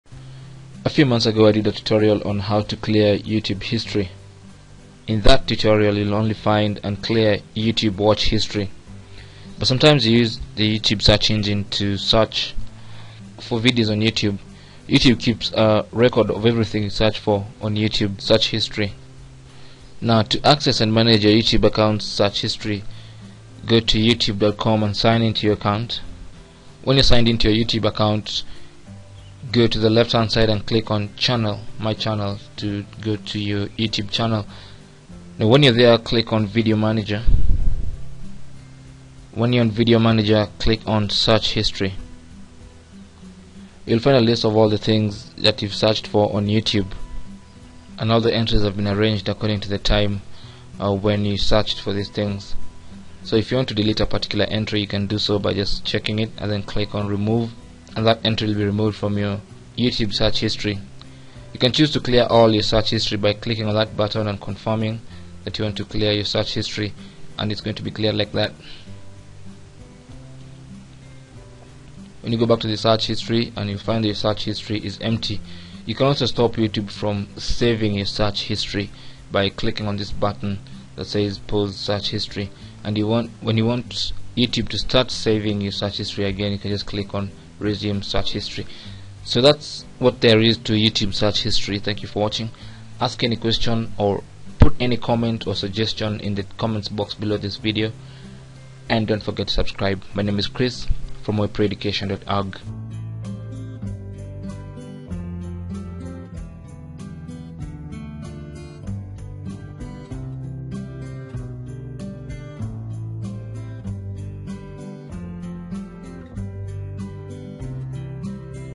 How To Clear YouTube Search History
In this short video tutorial, you will learn how to delete youtube search history

As you search Youtube for videos, channels and playlists, Youtube is keeping a record of everything you search for. Learn how to manage the information Youtube is keeping about you.

A nfew months ago I did a video on how to clear Youtube history. If you follow that video you will clear your Youtube watch history. But Youtube remembers much more that you watched. It keeps information about the searches you have made on its search engine.

To access and manage your YouTube account's search history:
Go to video manager as shown in the video.
Click on search history.

You will find a list of all the things you have searched on Youtube.
You can delete single or several entries of your search history as shown in the video.
You can also clear all your search history and stop YouYube from saving your search history.